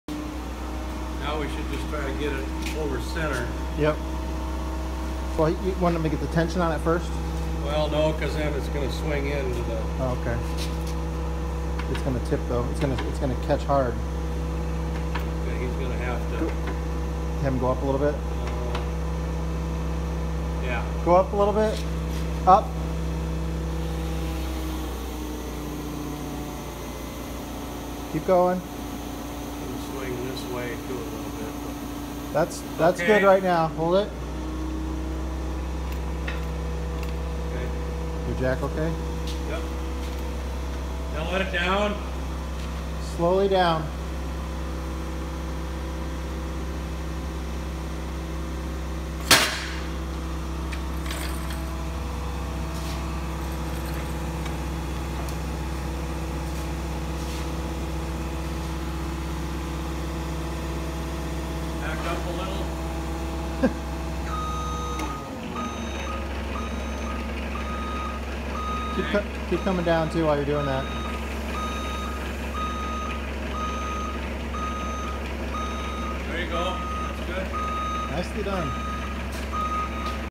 Creative way to roll the engine over
It worked just fine to rotate the engine Detroit diesel 671